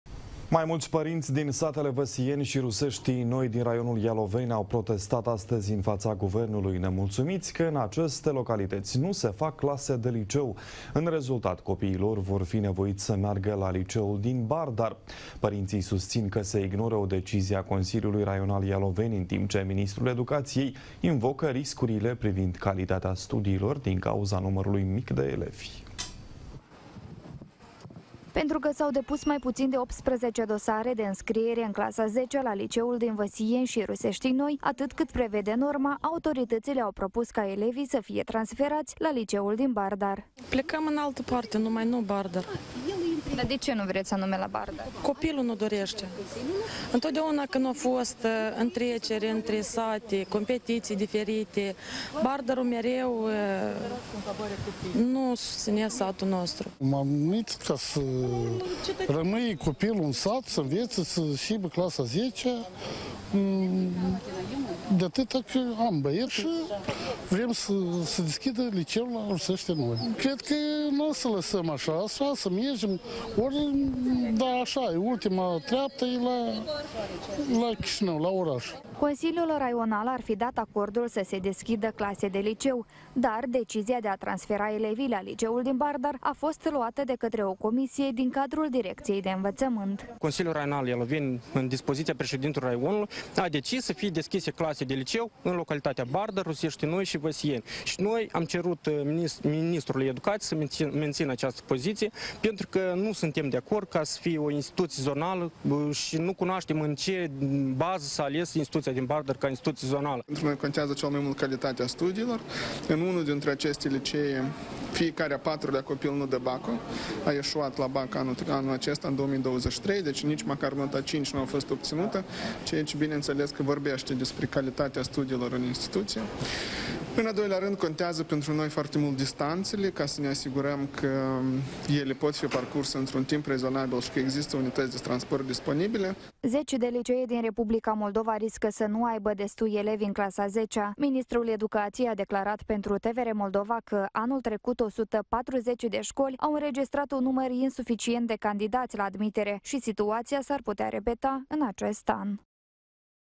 Mai mulți părinţi din satele Văsieni şi Ruseştii Noi au protestat la Guvern
Mai mulți părinţi din satele Văsieni şi Ruseştii Noi au protestat azi în faţa Guvernului, nemulţumiţi că in în aceste localităţi nu se fac clase de liceu. În rezultat, copiii lor vor fi nevoiţi să meargă la liceul din Bardar. Părinţii susţin că se ignoră o decizie a Consiliului raional Iaoveni in timp ce ministrul Educaţiei invocă riscurile privind calitatea studiilor din cauza numărului mic de elevi.